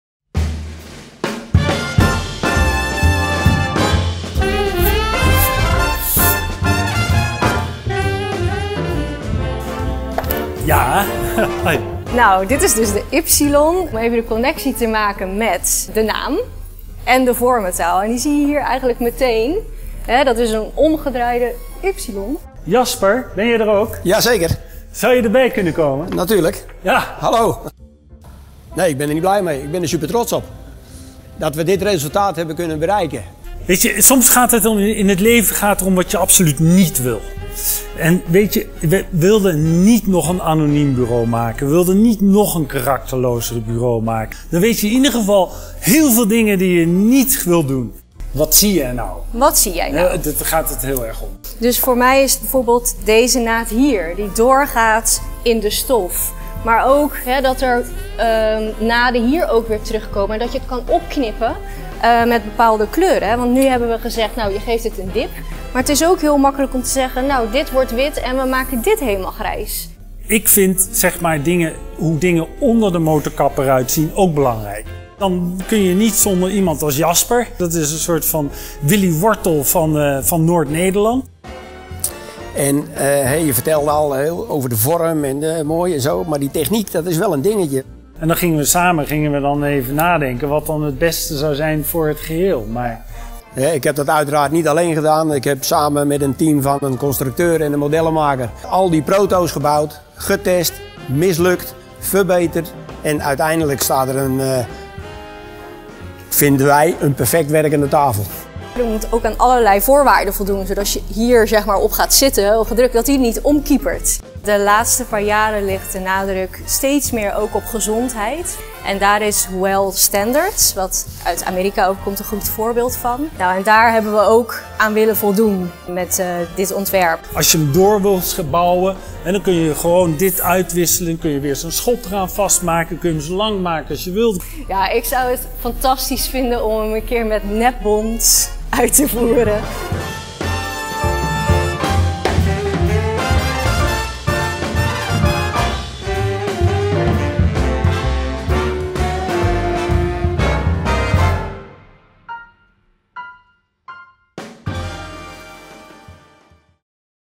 Ypsilon - Een compleet flexibel en schakelbaar bureausysteem, voor zowel zittend als staand werken.
uniek design | alle functies in één geïntegreerd design | akoestische tussenwand | zittend en staand werken | kabelmanagement in het frame

Rustig inrichtingsbeeld
Kiezen voor Ypsilon is vooral kiezen voor esthetiek zonder in te boeten aan functionaliteit.
De technische keuze om de bladen op te hangen aan een centrale kolom zorgt voor een nieuw archetype. Het blad zweeft optisch ten opzichte van het onderstel doordat er geen directe ondersteuning onder het blad zichtbaar is. Het  rustige beeld blijft behouden ook al richt je de werkplek volledig in.

Alle Ypsilon-werkplekken zijn elektrisch verstelbaar tot stahoogte, voorzien van geïntegreerde akoestische panelen en een handig geplaatste kabelgoot die aan beide zijden goed toegankelijk is. Design, functie en ergonomie gaan perfect samen.

Ypsilon is gerealiseerd in creatieve samenwerking met FOKLAb.